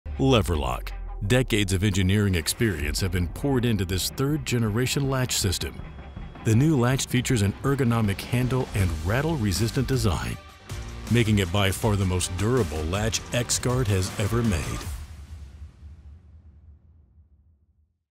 Ex-Guard LEVRLock NEW G3 Feature
After millions of miles traveled by trucks protected by Ex-Guard, we've learned and evolved. Introducing LEVRLock, a new feature of our Generation 3 guards.